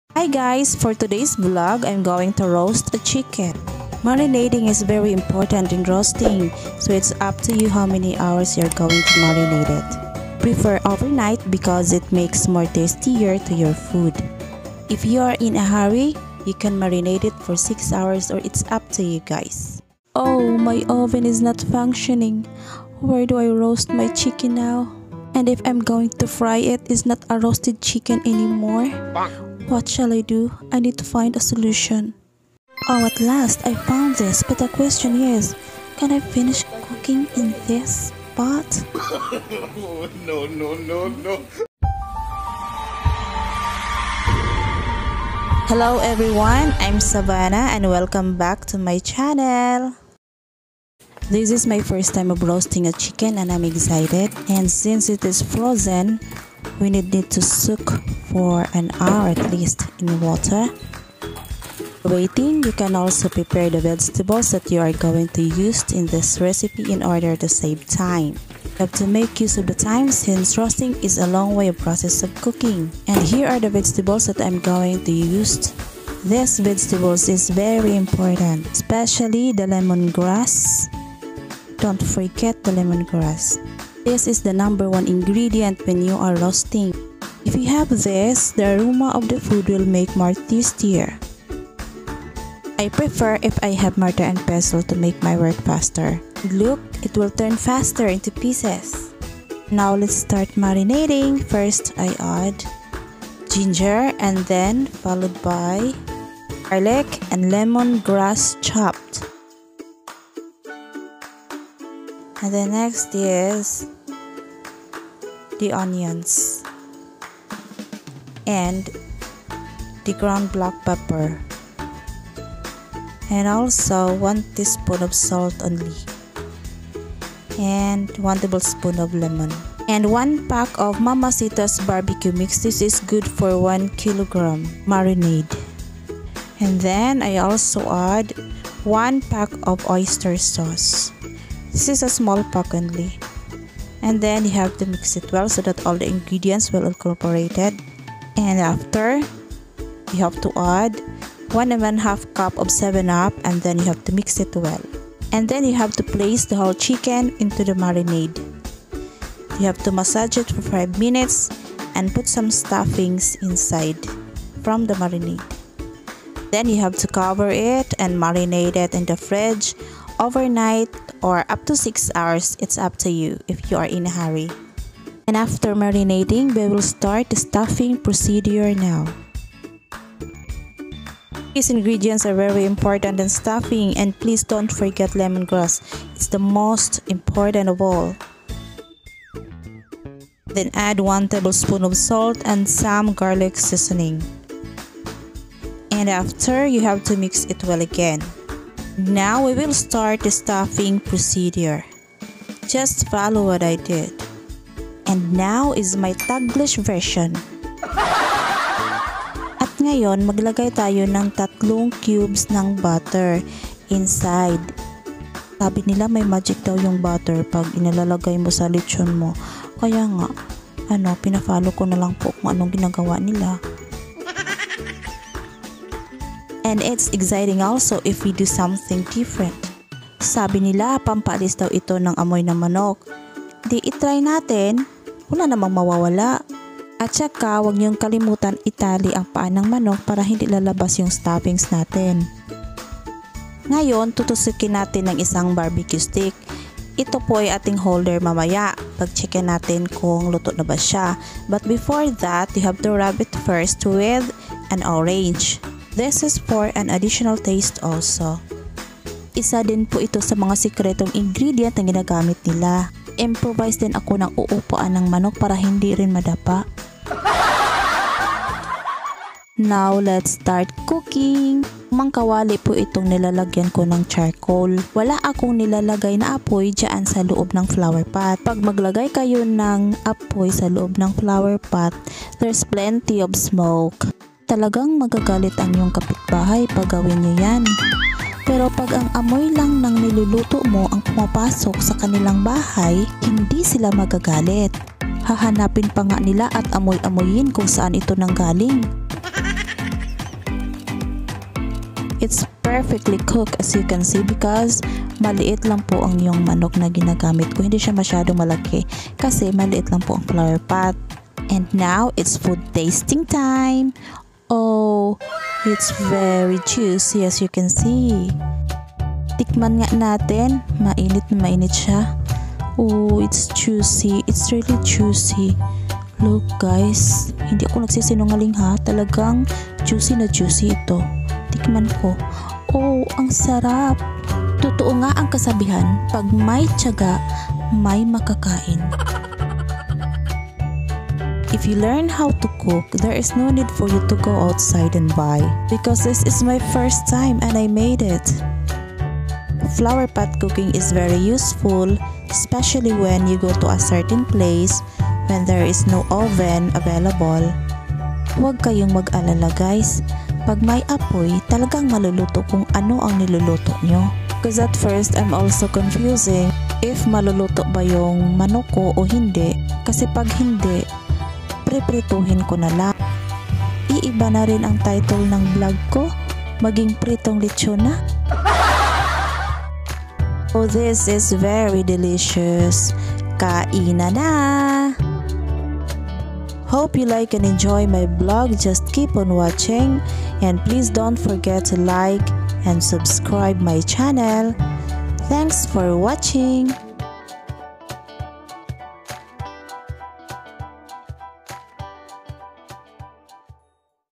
VLOG 05 - HOW TO ROAST CHICKEN IN A FlOWER POT | by Savanah
This is a method of cooking technique by using a flower pot as a DIY oven for roasting. It's an amazing experience !